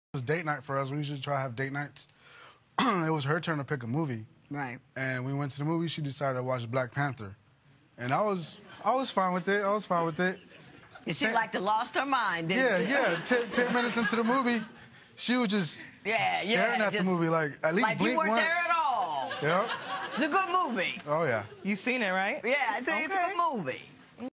Wakanda Fever!
See how her adoration for black men makes him insecure... even at the movies.